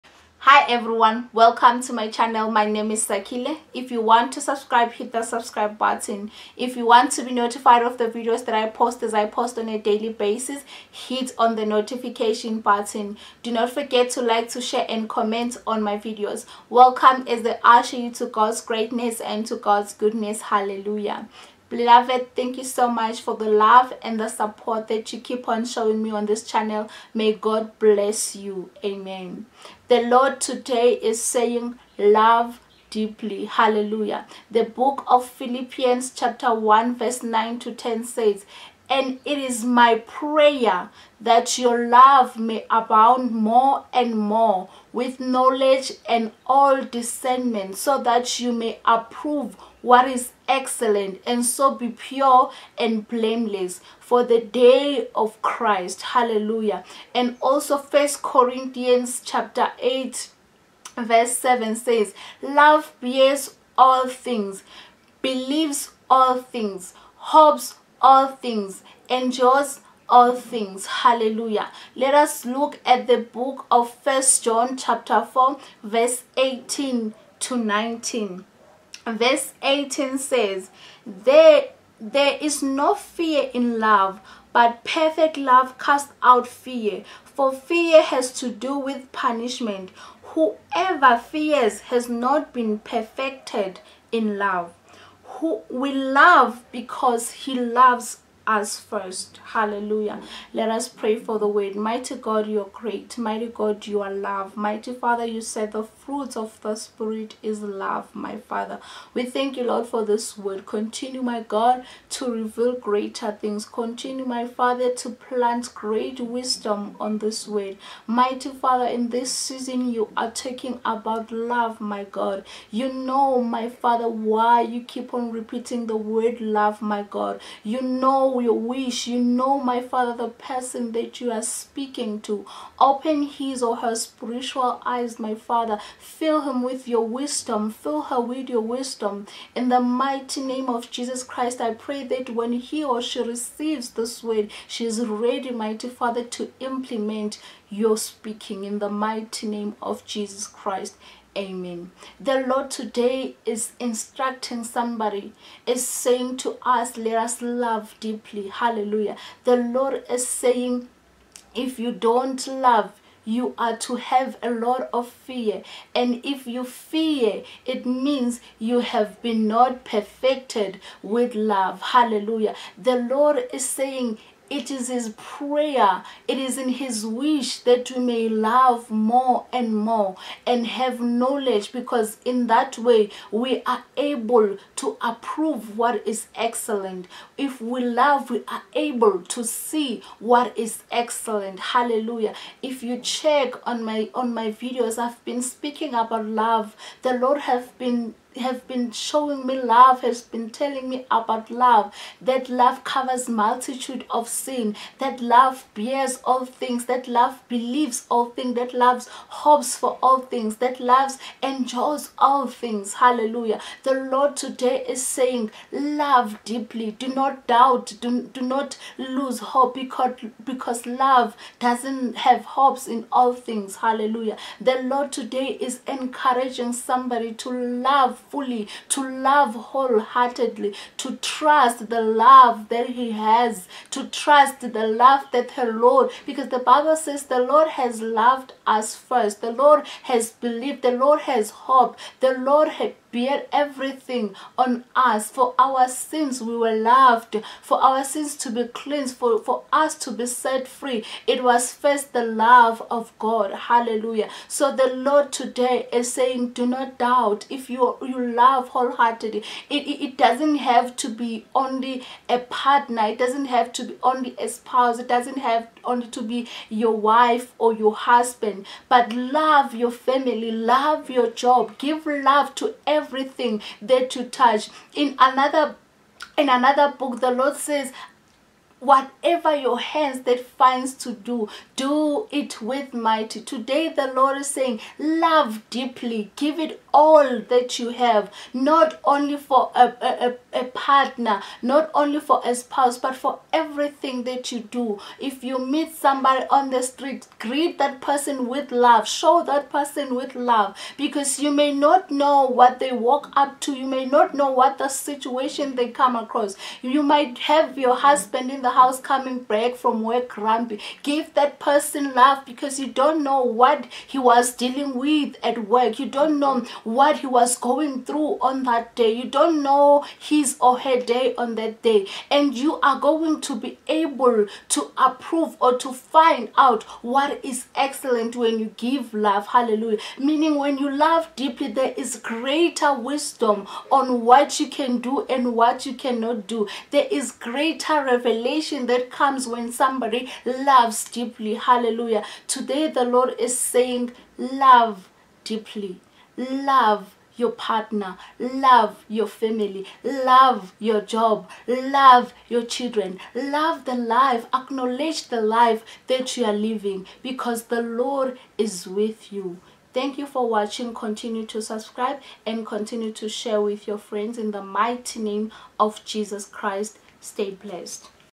Prophetic Word //💞💯💍He will require you to Love Deeply 💞💕💍👰
It is in His prayer that you may  more so that you may approve what is excellent.

Hatred can cause stress, Depression, stagmentation but Love covers all multitudes of sin.

For prophetic Word, please pray and ask for God's wisdom and guidance.

For prayer requests, suggestions and counseling email